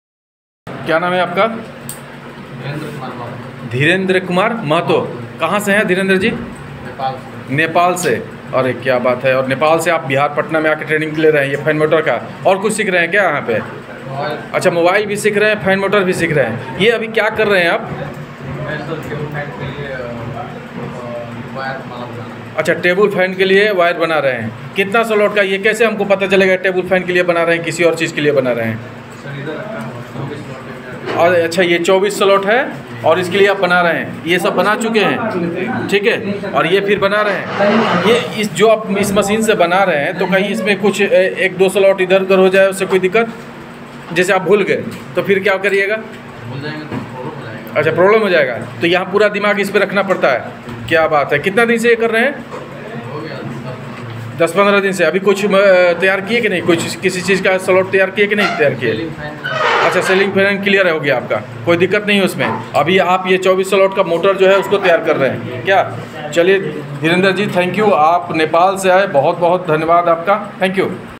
24slot tebal fan motor winding training Nepal ka bacha Bihar me jabrjast training le rahe hai
Bihar Technical Training Center || Repair सीखने वाले छात्र क्यूँ  है  इतने खास

सीखने के लिए कॉल करे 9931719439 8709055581

OUR COURSES:-

Our Quries